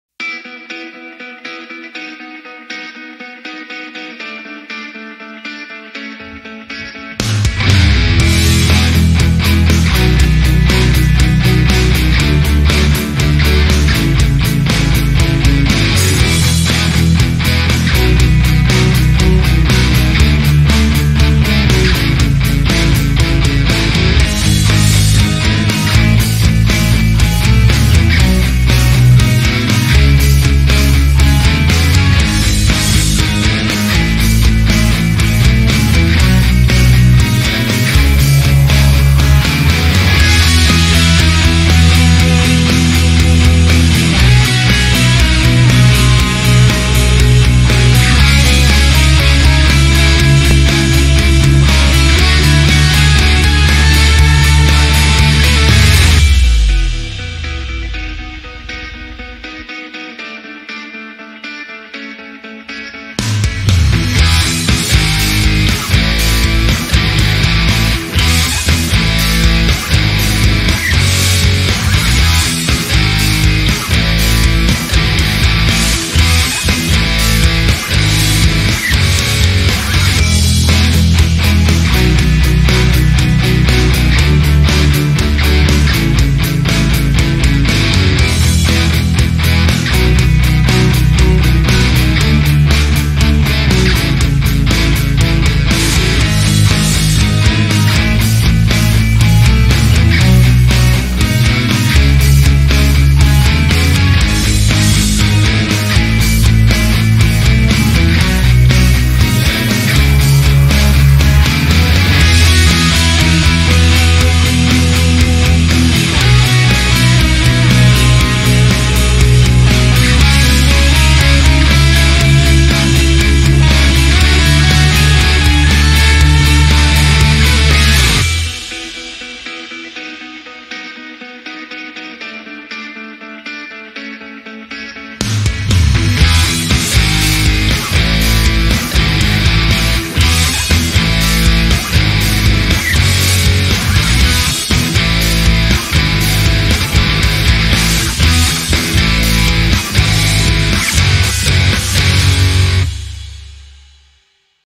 Workout Music, get energized with this pump up hard beat music
filled yourself with this workout music and go all out and knock all those dumbbell and weight, run and jump.